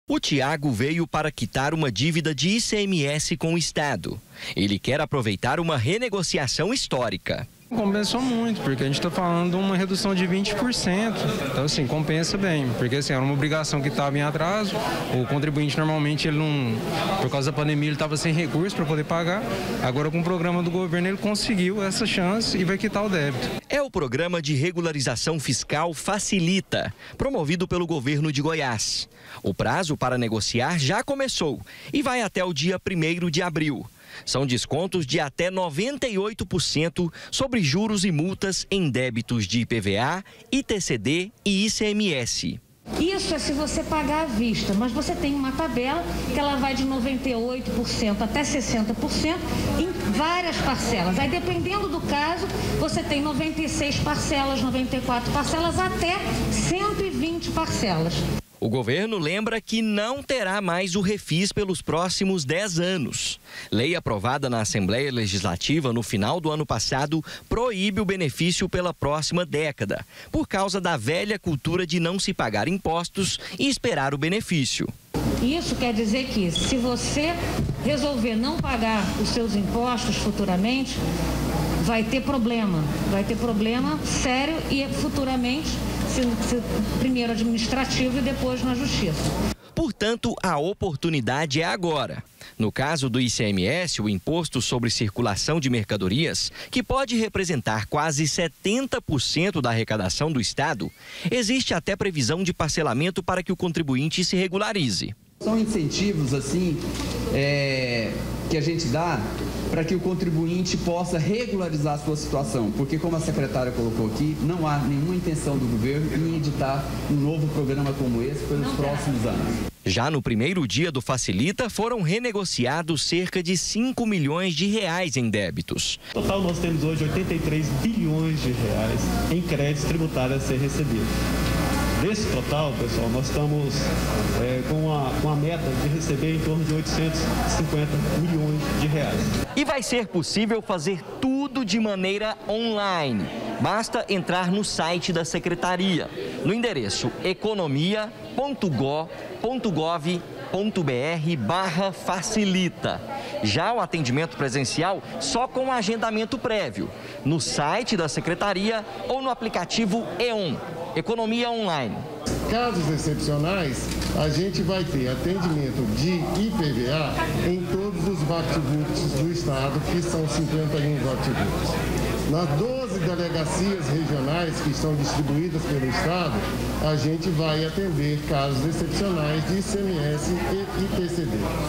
REGULARIZAÇÃO FISCAL: PROGRAMA VAI FACILITAR RENEGOCIAÇÃO DE DÍVIDAS DO IPVA E ICMS
JÁ ESTÁ VALENDO O PROGRAMA DE REGULARIZAÇÃO FISCAL DO GOVERNO DO ESTADO. ENTRE OS DÉBITOS QUE PODEM SER NEGOCIADOS ESTÃO IPVA E ICMS.